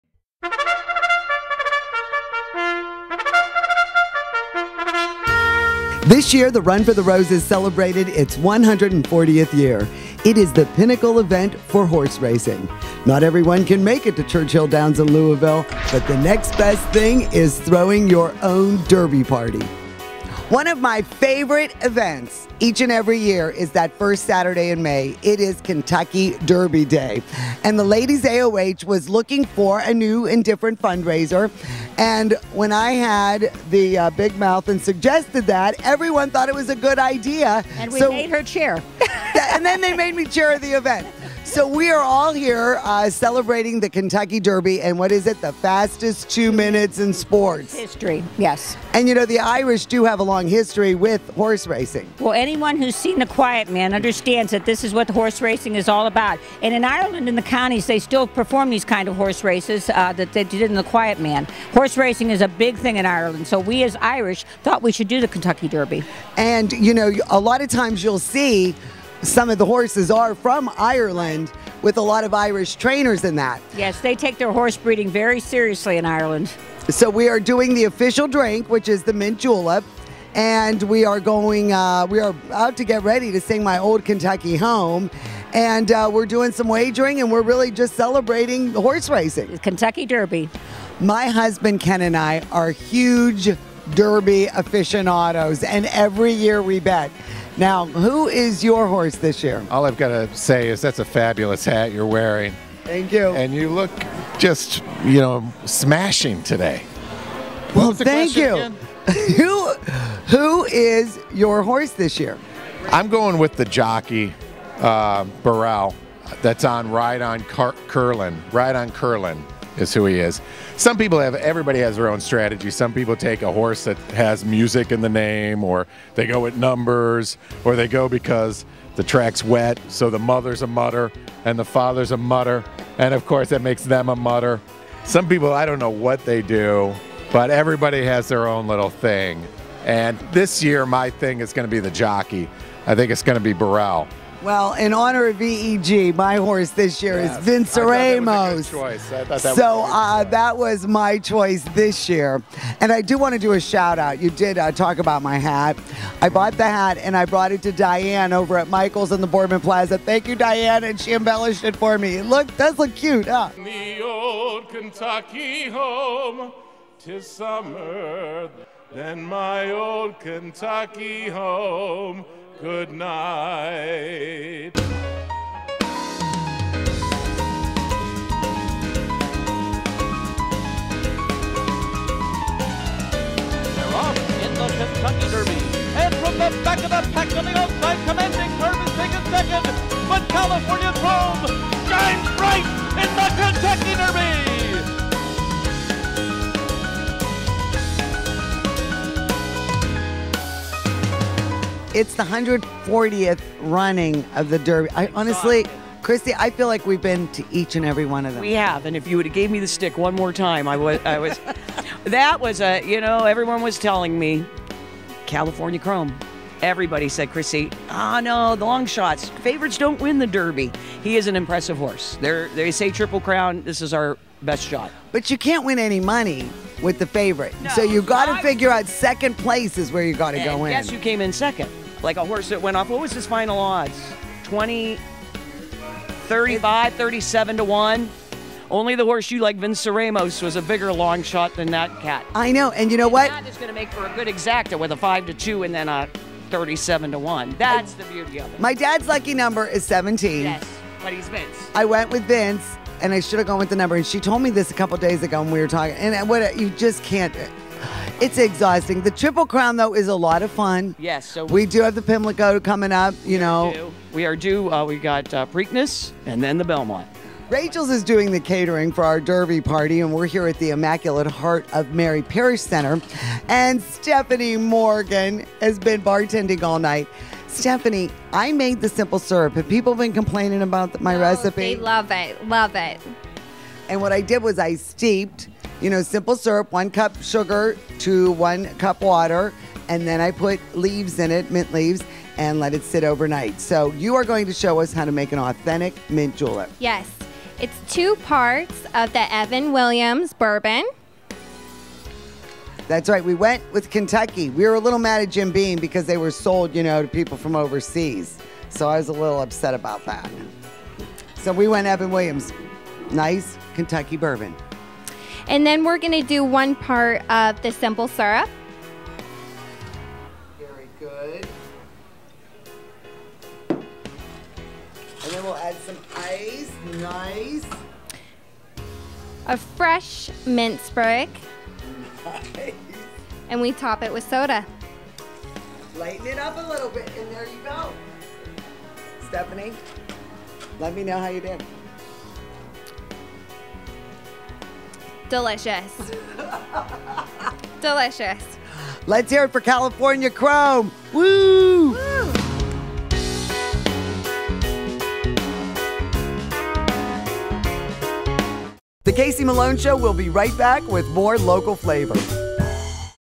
LAOH Derby Day Benefit
Each week Casey Malone serves up the best our region has to offer. The Kentucky Derby Party thrown by the LAOH in the Mahoning Valley was a fun bash. Authentic Mint Juleps, horse wagering, gorgeous hats and of course the big race!